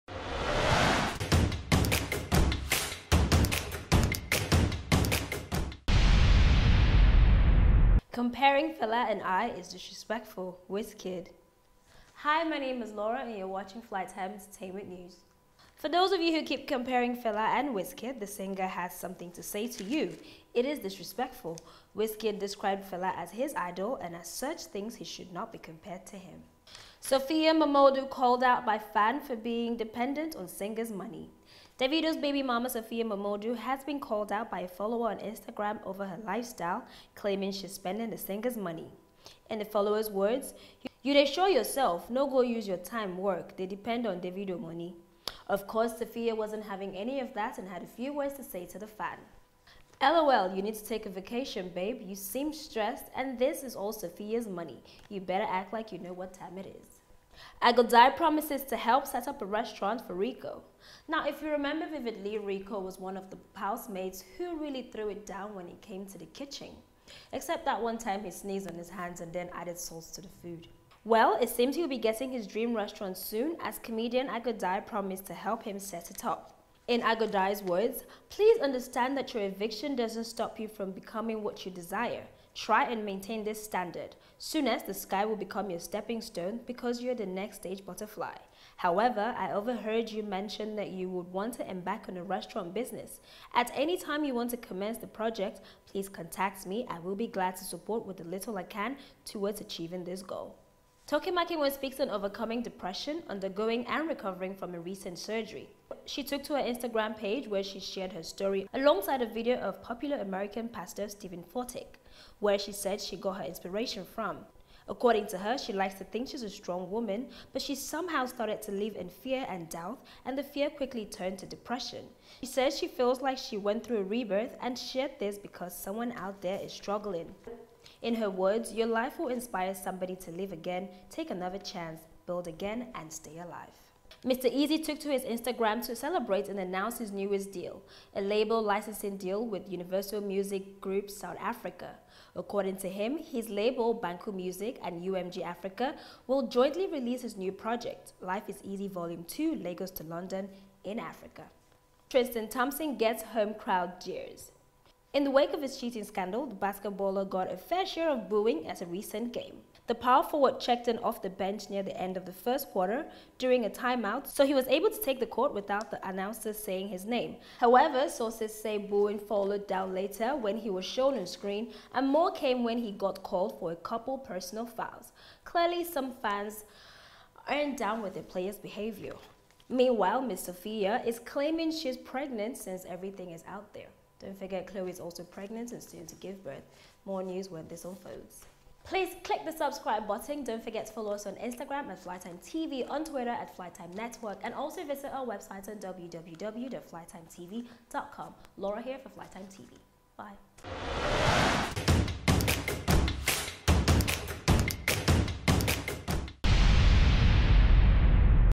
Flytime TV: It Is Very Disrespectful To Compare Fela & I - Wizkid Says
Popular Nigeran artiste, Wizkid has said it is very disrespectful to compare him with a legend like Fela.
- Sophie Momodu claps back at guy who said she is living on Davido's money.
- Nigerian  I go Dye promises to